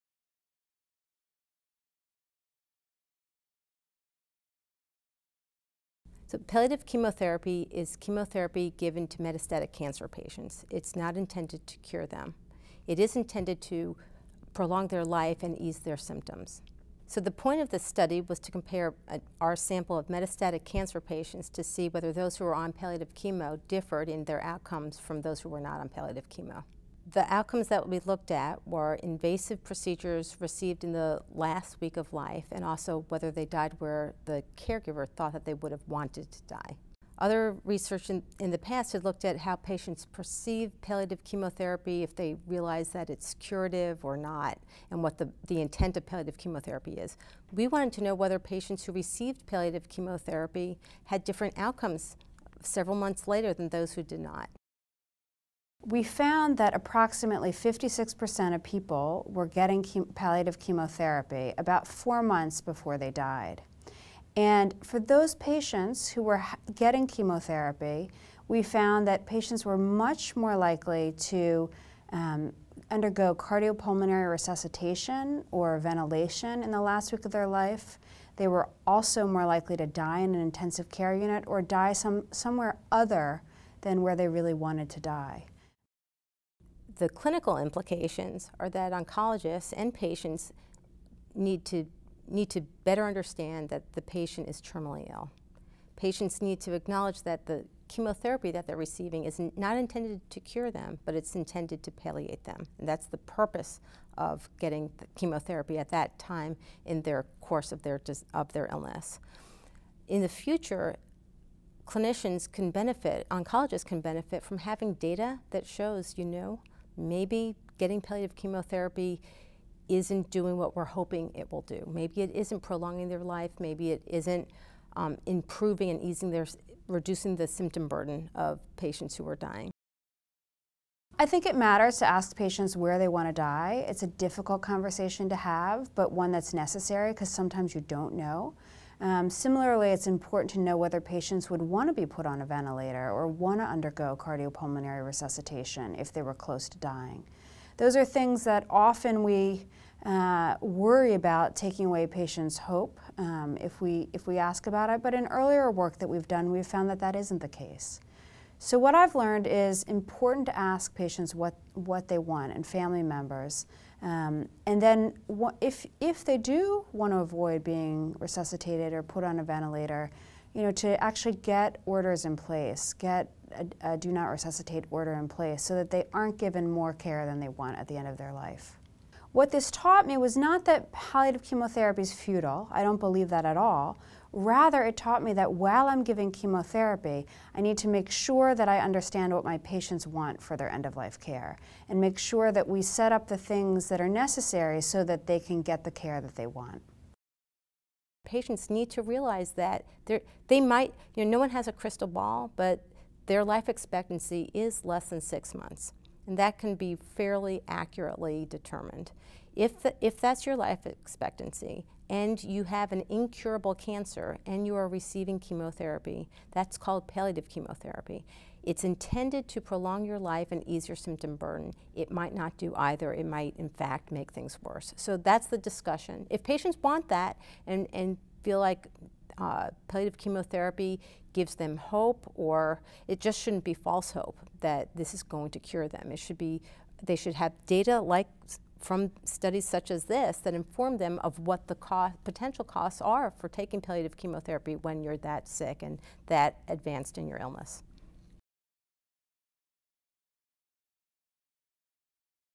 Prospective Cohort Study of Palliative Chemotherapy
Chemotherapy is used to treat metastatic cancer, with the goal of palliating symptoms and improving survival. Between 20% and 50% of patients with incurable cancers receive chemotherapy within 30 days of death, despite growing concerns that it may not be effective.

Recently, the American Society for Clinical Oncology identified end of life chemotherapy as one of the "top five" practices that could improve patients' care and reduce costs, if stopped. Deciding when to discontinue chemotherapy is often challenging, however, given limited data on whether chemotherapy is helpful or harmful in the final months of life.

In this study, the authors set out to determine whether the receipt of chemotherapy among terminally ill cancer patients months before death was associated with patients' subsequent intensive medical care and place of death.